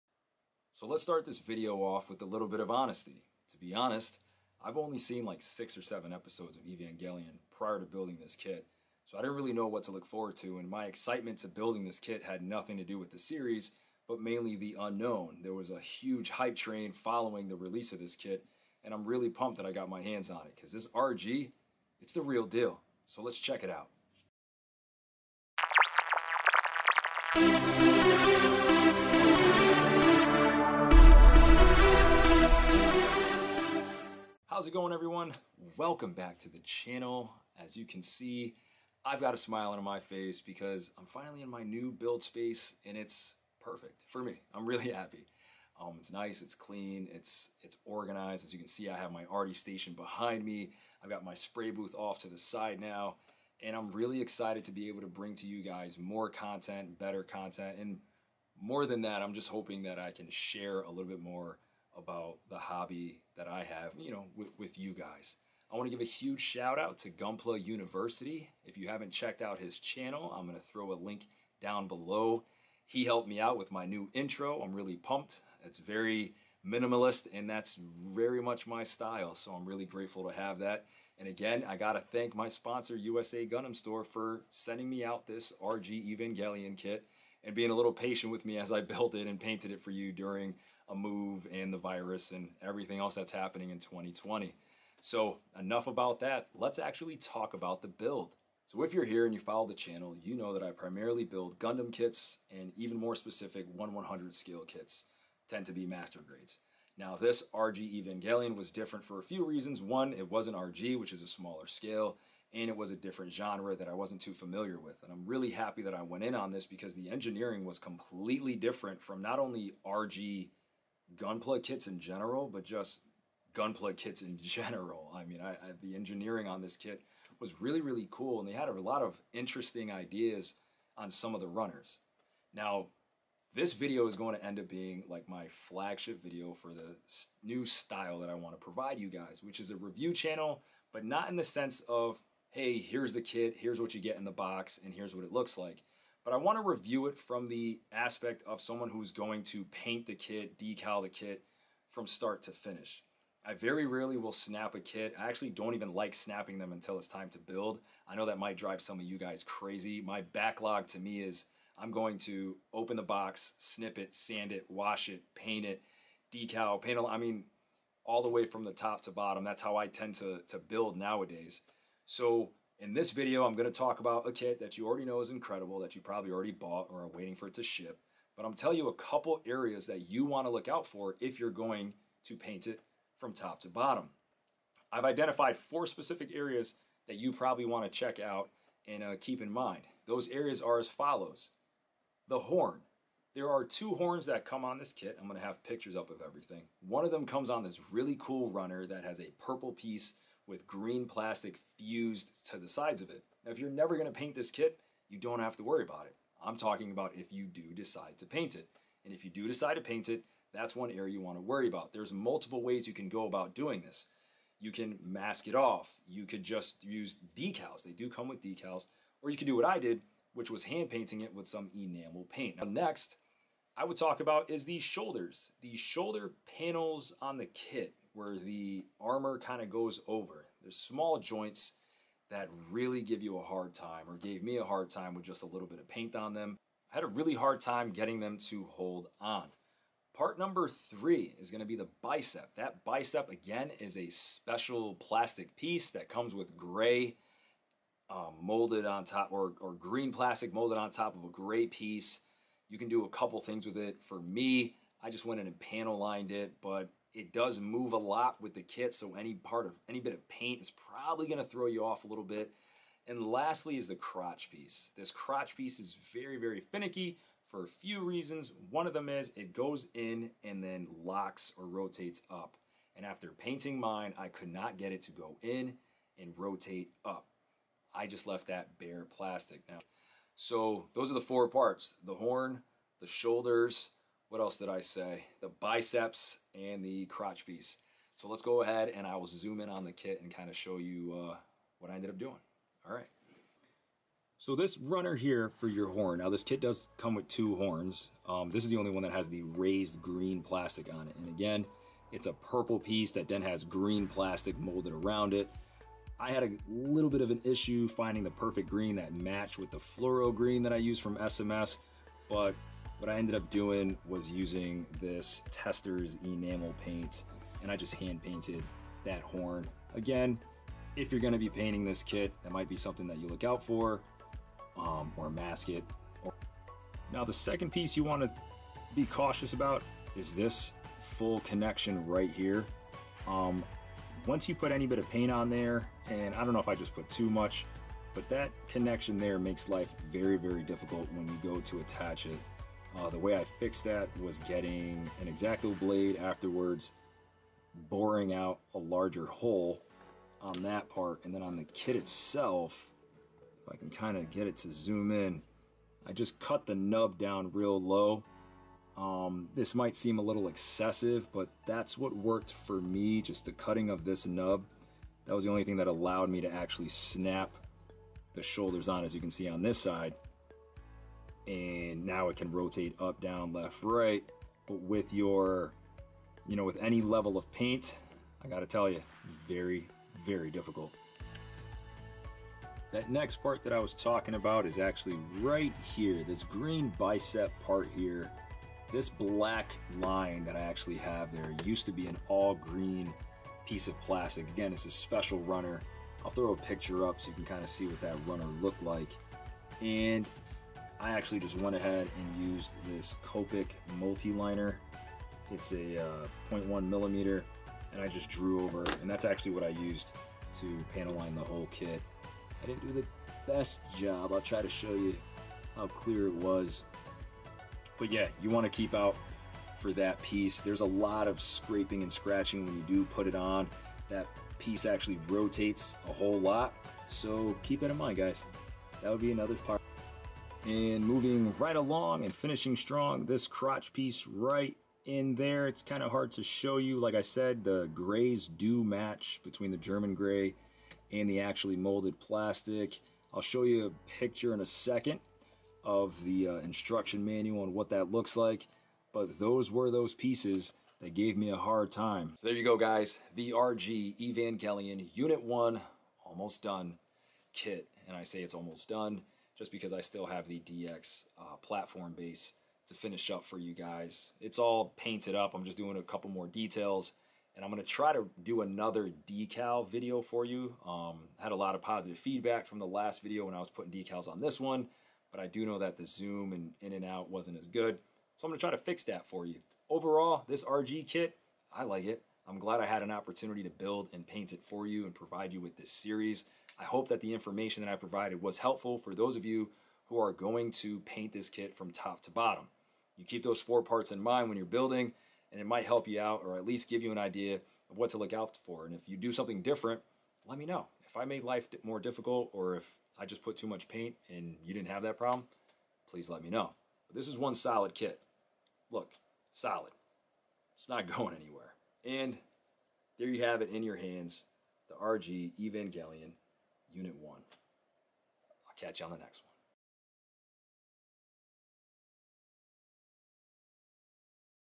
RG Evangelion Unit-01 PT.3: Tips for Painters
While working on this build, I identified 4 areas on the kit that you should keep in mind when it comes to painting. Overall, this is one really solid kit, but you already knew that. I hope this video helps you out when it comes time to paint and final assembly. Hope you enjoy.